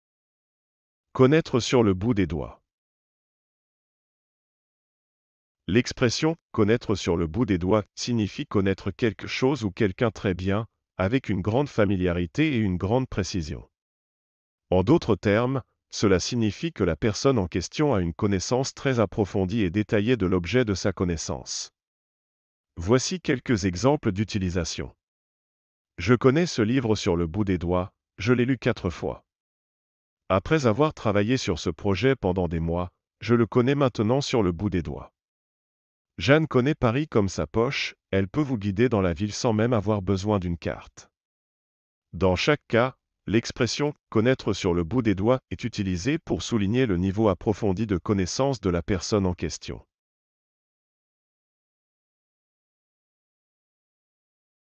Connaître sur le bout des doigts
L'expression "Connaître sur le bout des doigts" signifie connaître quelque chose ou quelqu'un très bien, avec une grande familiarité et une grande précision. En d'autres termes, cela signifie que la personne en question a une connaissance très approfondie et détaillée de l'objet de sa connaissance.
Voici quelques exemples d'utilisation :
- Je connais ce livre sur le bout des doigts, je l'ai lu quatre fois.
- Après avoir travaillé sur ce projet pendant des mois, je le connais maintenant sur le bout des doigts.
- Jeanne connaît Paris comme sa poche ; elle peut vous guider dans la ville sans même avoir besoin d'une carte.
Dans chaque cas, l'expression "Connaître sur le bout des doigts" est utilisée pour souligner le niveau approfondi de connaissance de la personne en question.